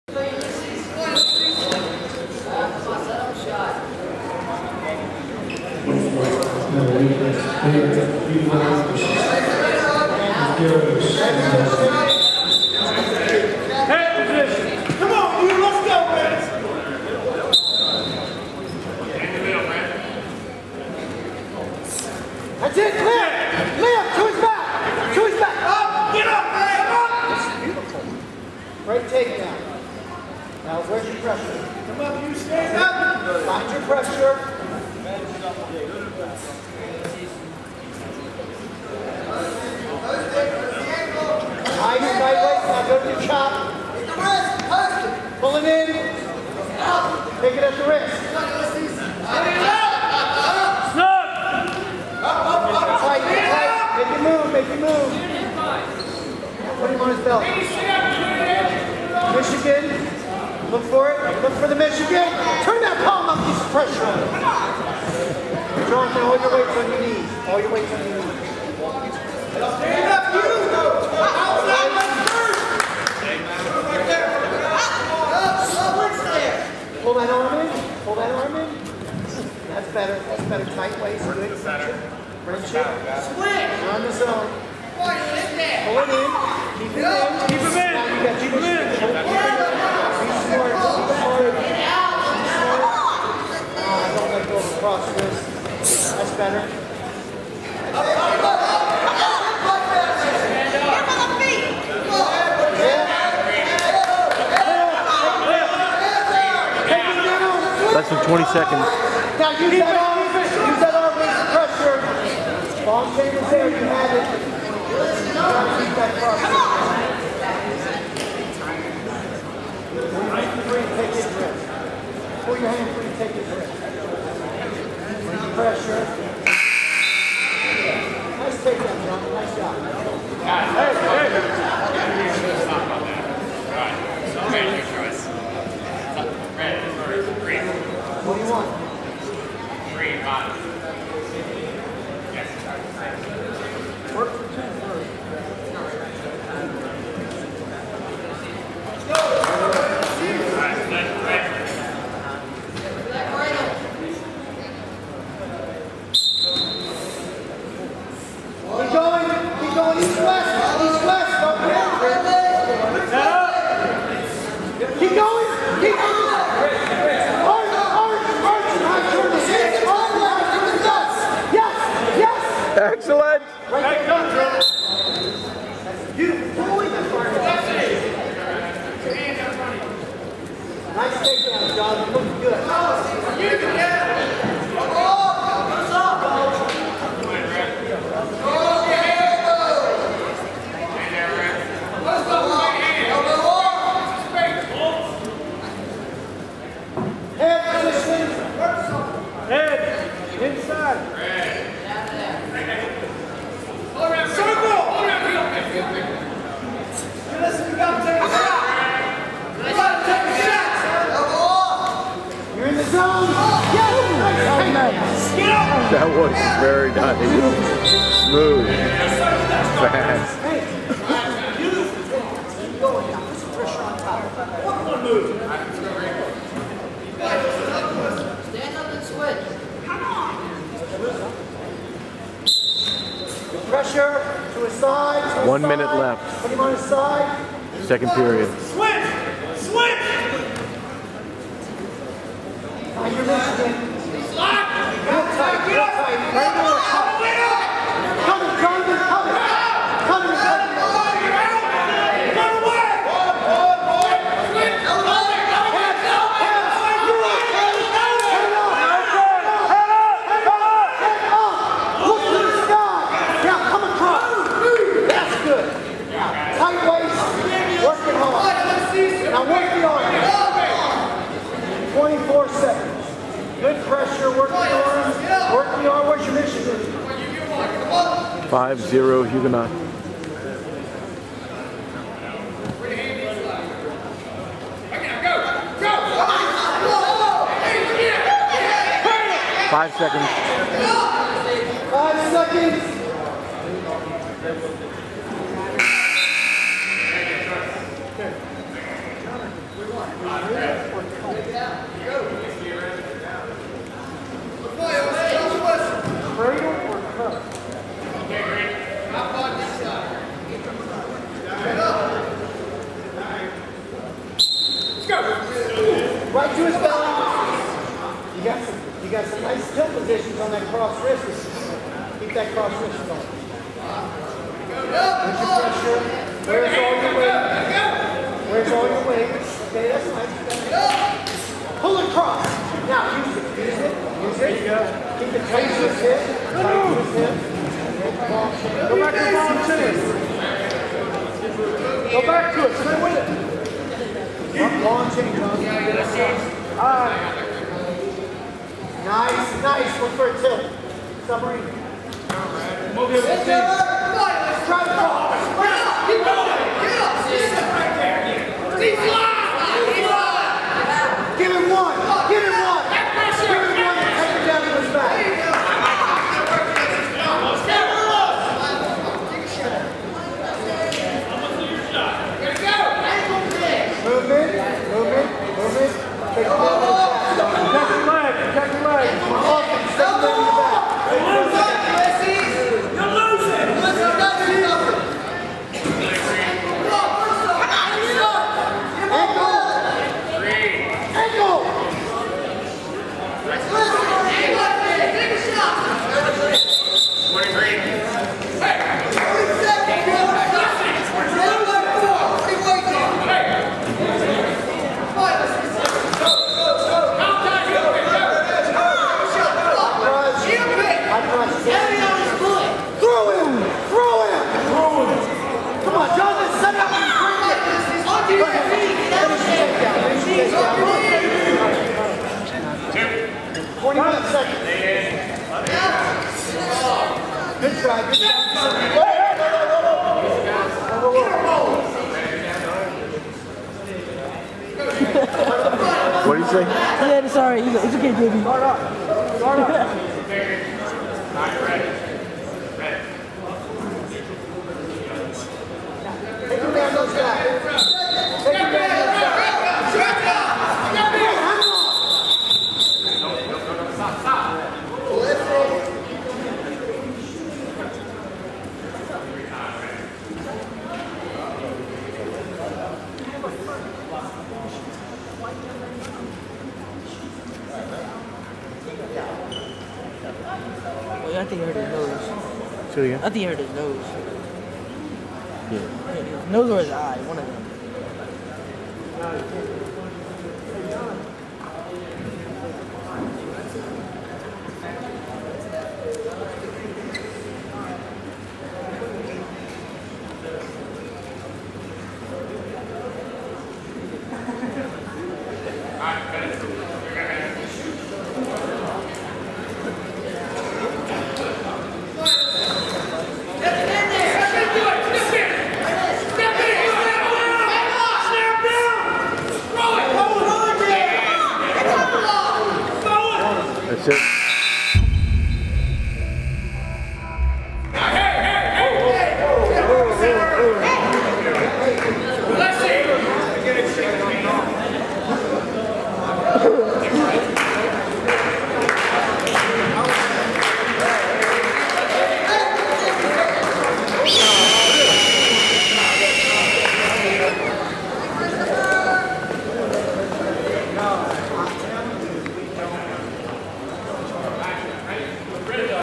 Jonathan Huguenot 2 Godwin Tri Meet Dec 21, 2011
High School Wrestling Team Tri-Meet with Varina High SchoolHuguenot  and Godwin High School
Jonathan Barrett 145 lb. Match